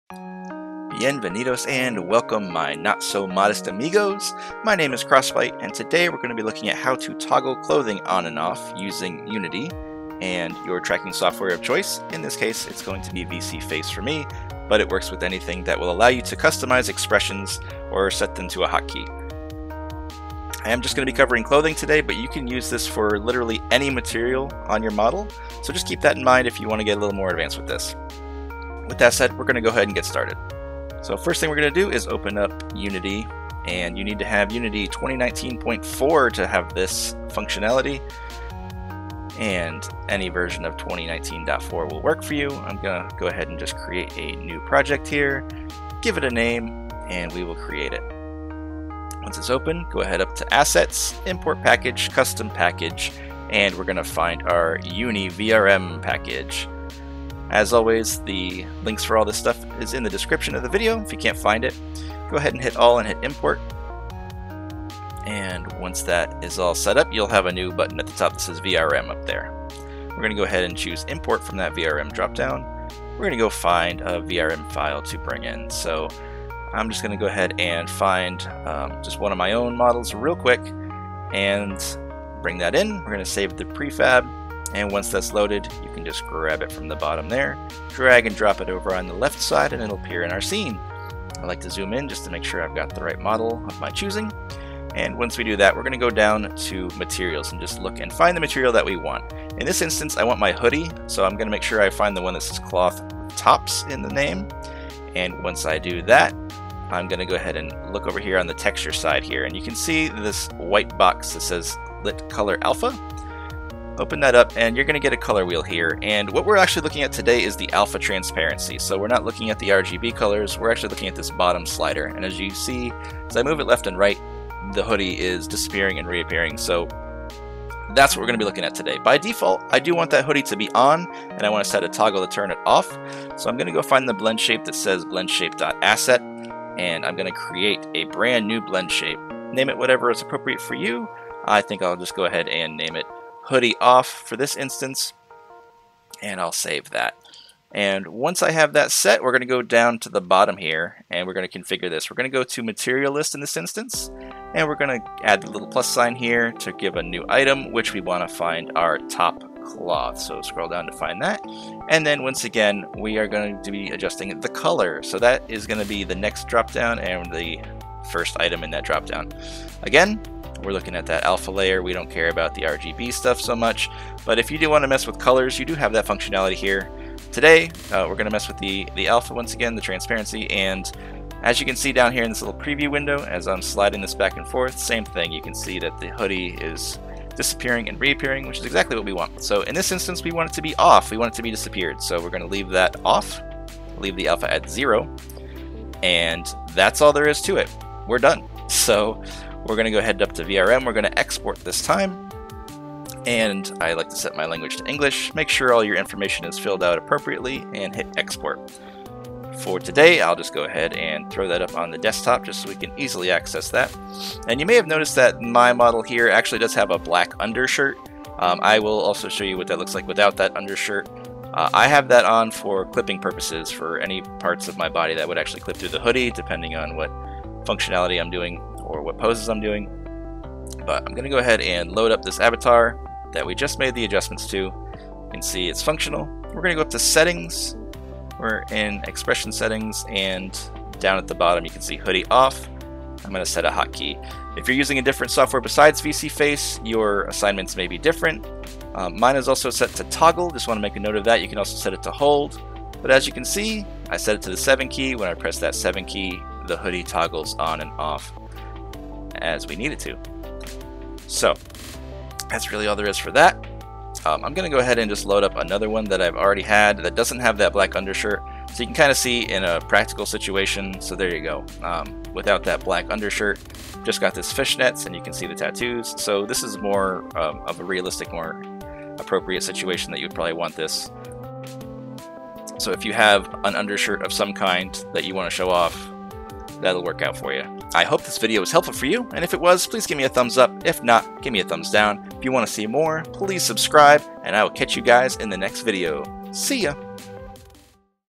Unity Tutorial: Take Off Your Clothes
In this tutorial we utilize the alpha transparency to add or remove clothing from your VRoid/VRM model. This effect can be used on any material that is assigned to your model. You can also to the reverse, by starting with the material turned off by default and enabled using a hotkey. Please make sure to use Unity 2019.4 in order to utilize the UniVRM plug-in!